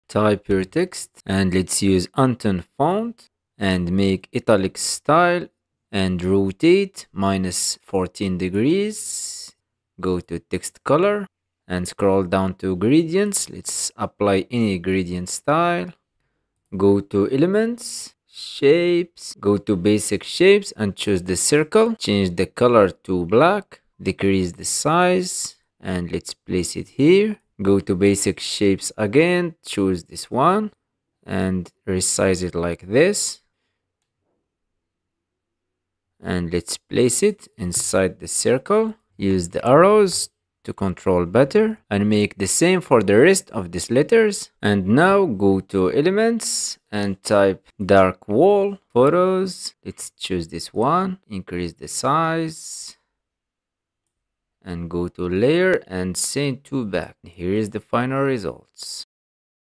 Canva Text Effect Tutorial: Create Stunning Hanging Text!
Learn how to create a professional Hanging Text Effect in Canva in just a few seconds! This quick Canva tutorial shows you how to design eye-catching text for thumbnails, posters, and social media. Perfect for beginners and creators looking to level up their Canva text effects.
Watch now and try it in your next design!